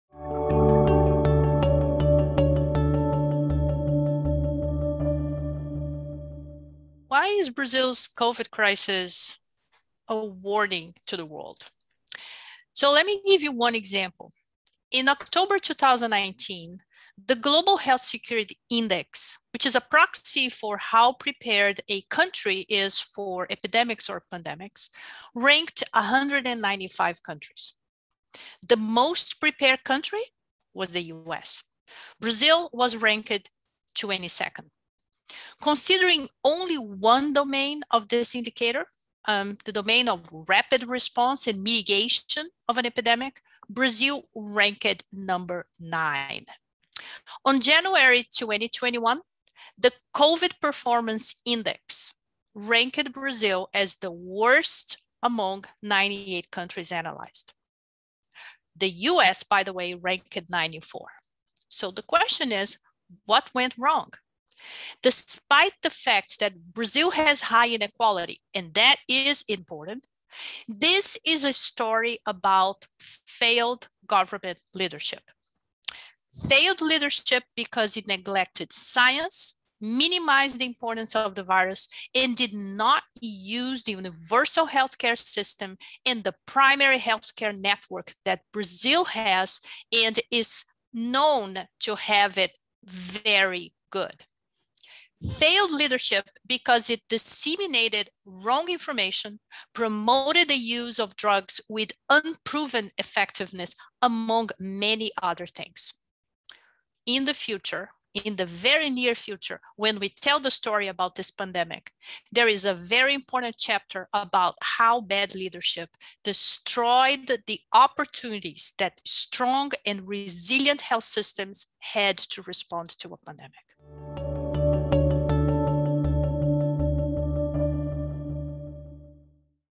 Marcia Castro: Why is Brazil’s COVID-19 crisis a warning to the world?
Marcia Castro, Chair of the Department of Global Health and Population at the Harvard T.H. Chan School of Public Health, discusses how Brazil's current COVID-19 crisis might provide a warning and response lesson for the rest of the world.